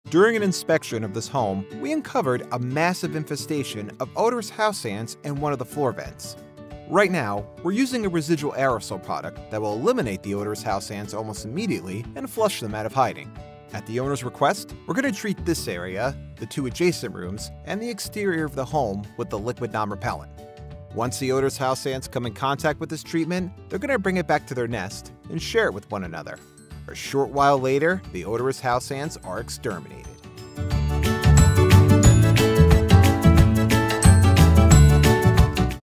Floor Vents Full of Odorous House Ants
No wonder the homeowner was smelling rotten coconuts in her dining room!